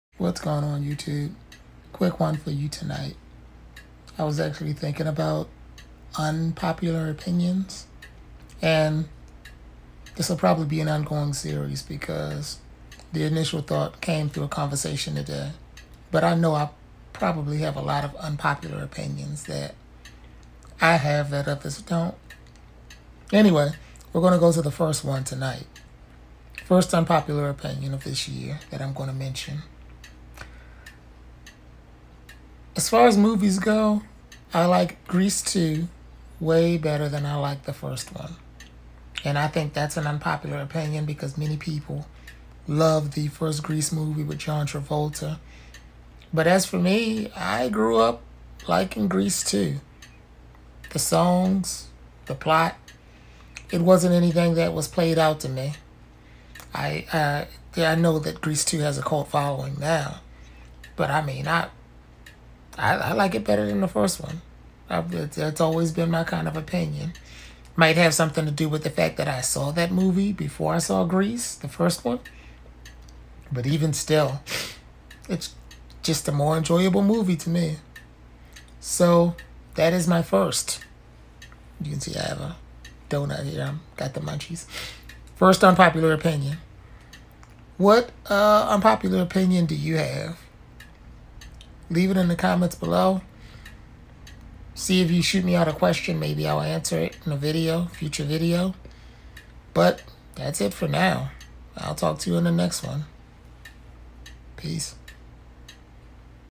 Day 155 Vlog- Unpopular Opinions #1
New little mini series that you will see based on things as I remember them. Share yours in the comments...